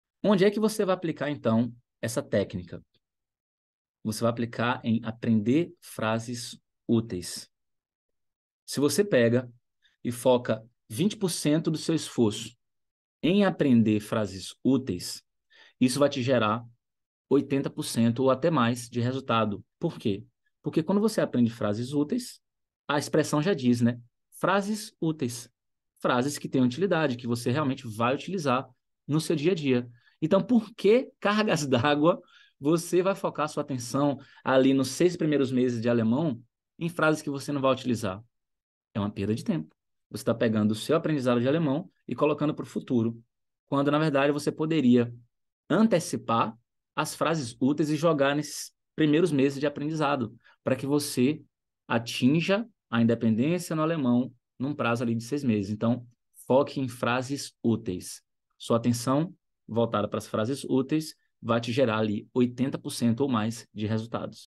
O que estudar para acelerar a comunicação em alemão?
Existem algumas coisas que são responsáveis pela maior parte dos resultados dos nossos projetos. Se você almeja falar o alemão do dia a dia em 6 meses, você precisa se atentar a esse e outros detalhes para alcançar seu objetivo.

É possível falar alemão, basta você querer!